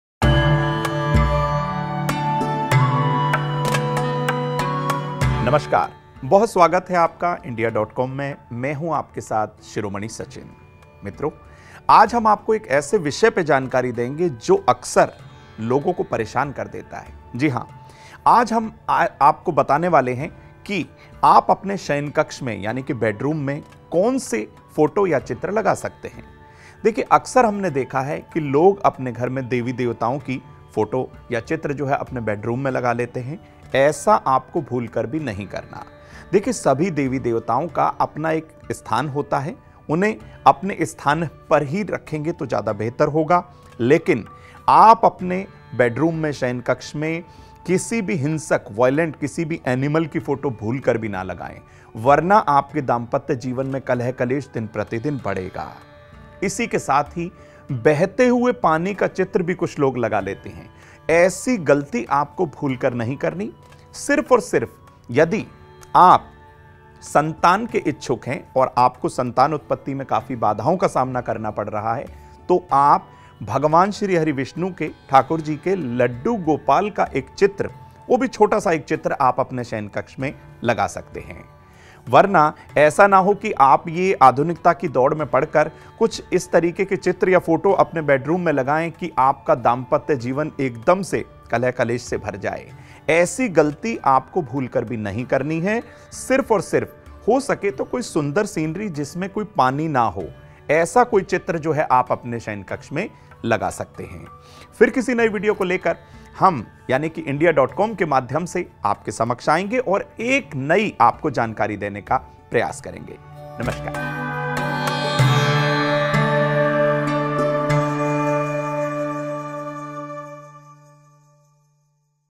बेडरूम में क्या देवी-देवता की तस्वीर लगानी चाहिए? Jyotish Upay | Astrology
बेडरूम में क्या देवी-देवता की तस्वीर लगानी चाहिए? Jyotish Upay
बेडरूम में क्या देवी-देवता की तस्वीर लगानी चाहिए? अगर हां तो किन किन की तस्वीर लगा सकते हैं, वीडियो में जानें ज्योतिष गुरु शिरोमणि सचिन से. बेडरूम में देवी देवताओं की तस्वीर न लगाएं. बेडरूम में हिंसक तस्वीर न लगाए इससे दाम्पत्य जीवन पर प्रभाव पड़ता है. बहते हुए पानी का चित्र न लगाएं संतान प्राप्ति के लिए भगवान लडडू गोपाल का छोटा चित्र लगाएं. सीनरी जिसमे पानी न हो, वो आप बेडरूम में लगाएं.